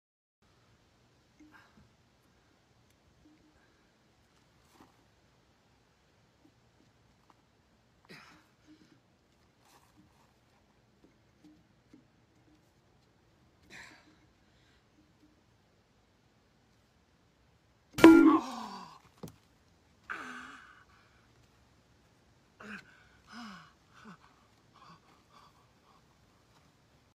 old man vs crowbar meme
ouch! that hurts i that's the video i added the bonk sound effect